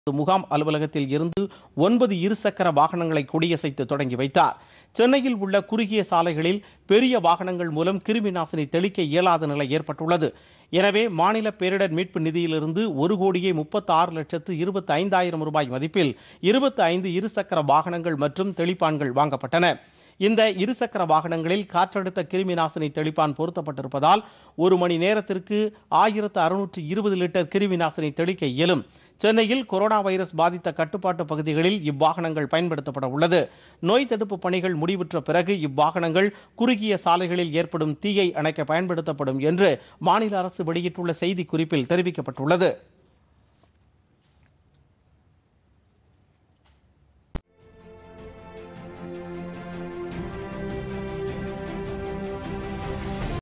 கிருமிநாசினி வாகனங்கள் அறிமுகம்
கிருமிநாசினி தெளிக்கும் இருசக்கர வாகனங்களை இன்று துவக்கிவைத்தார் முதலமைச்சர் எடப்பாடி பழனிசாமி.